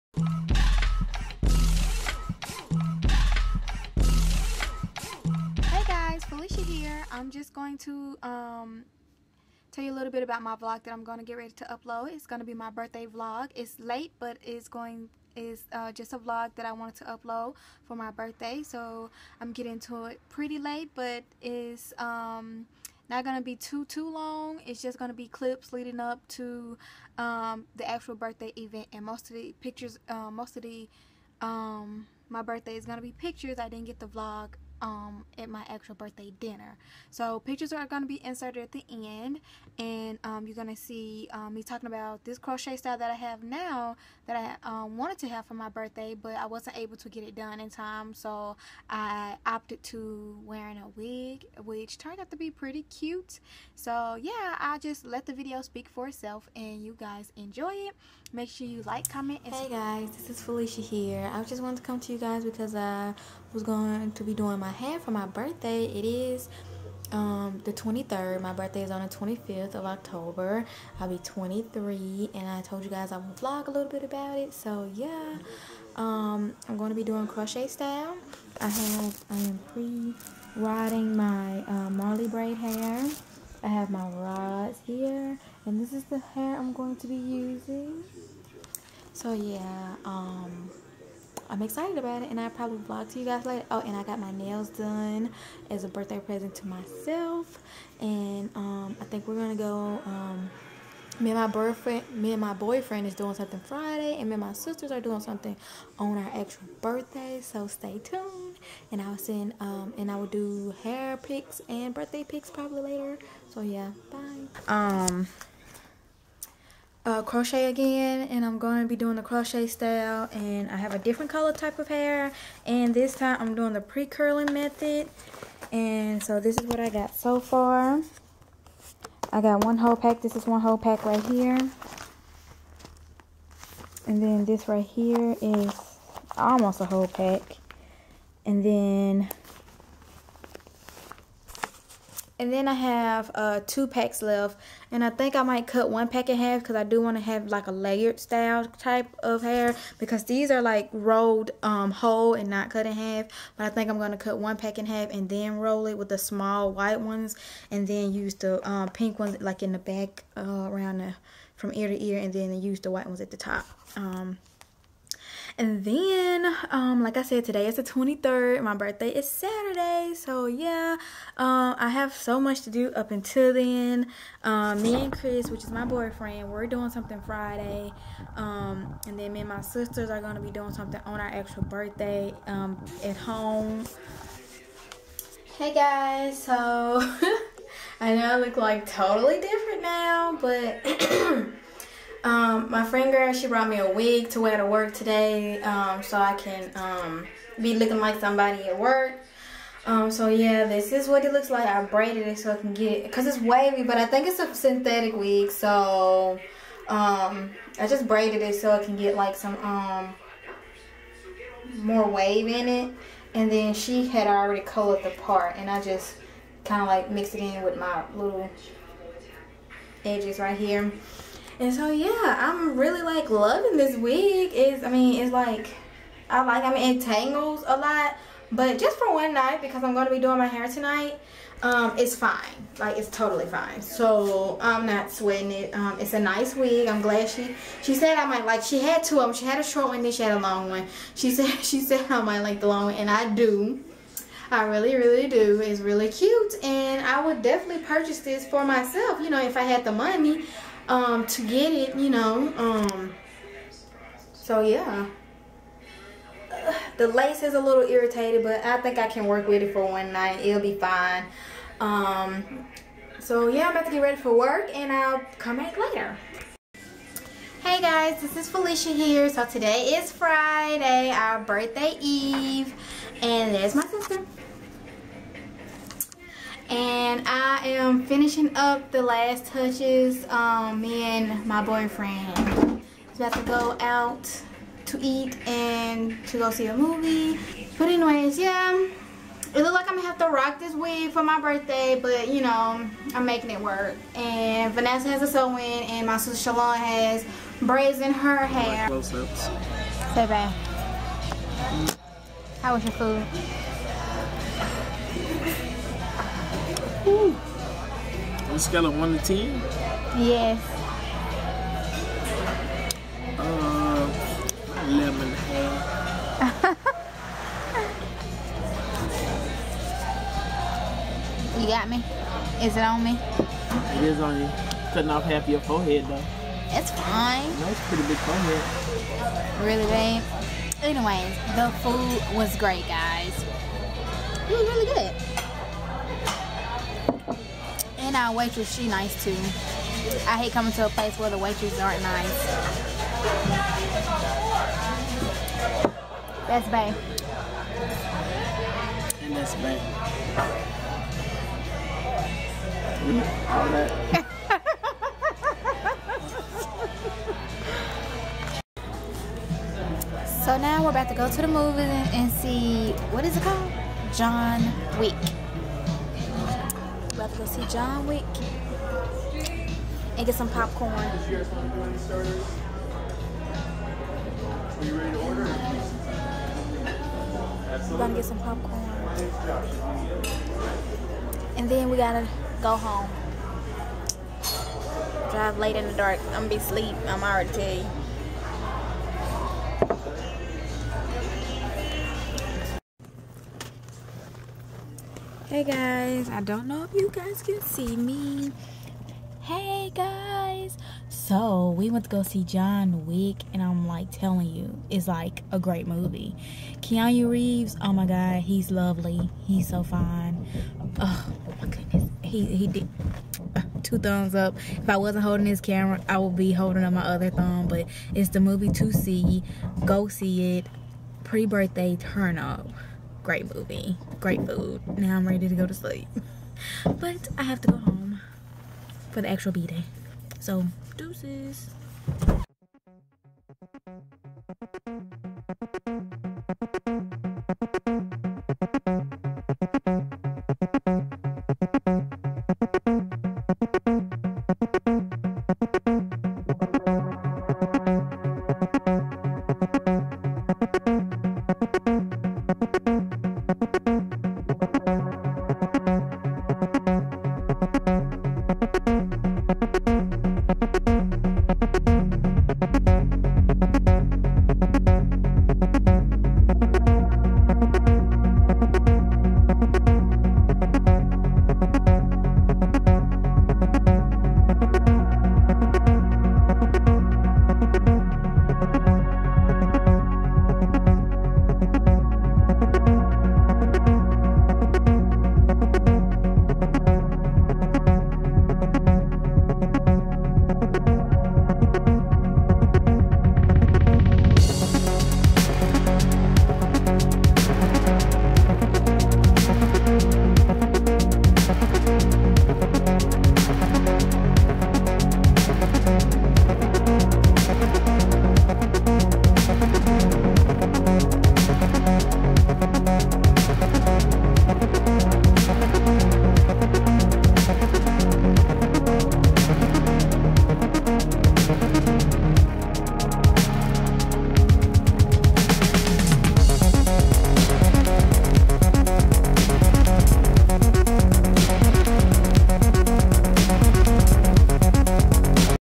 BIRTHDAY VLOG *October 25*
Lost flie
This is just a short vlog leading up to my birthday and there are only pics for our actual birthday because i didnt get to vlog at the dinner

Me and my triplet sisters turned 23 yrs old and we spent it with family and friends